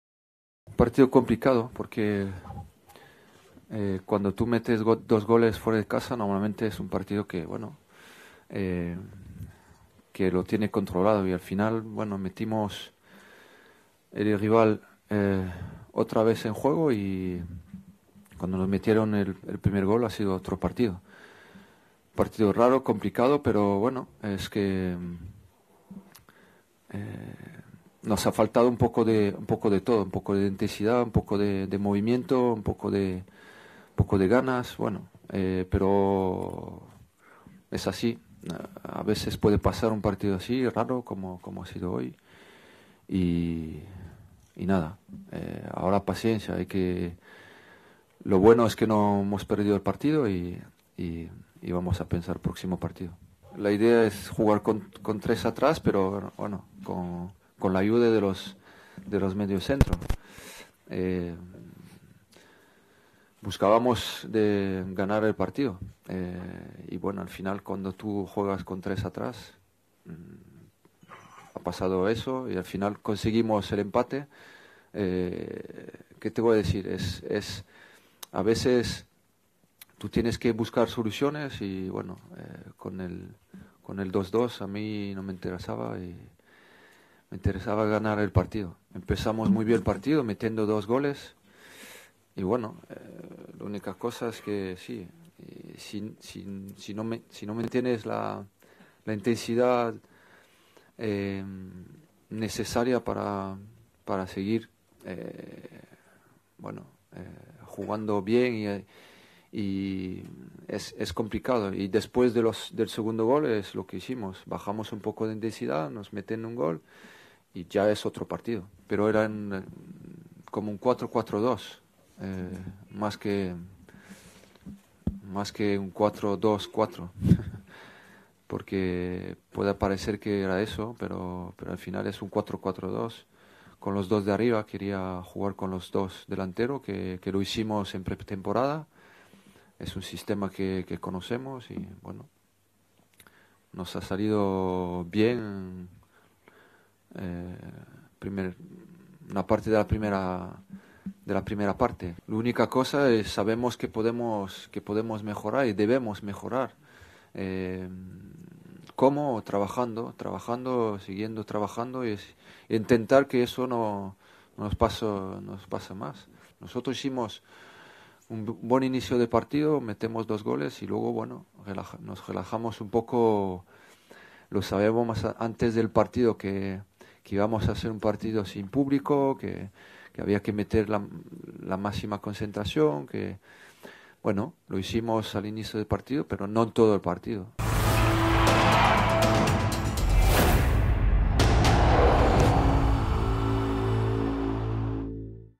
Zidane valoró el empate del Real Madrid en el campo del Legia de Varsovia
Zidane: "Después del 0-2 bajamos la intensidad, nos marcaron y fue otro partido"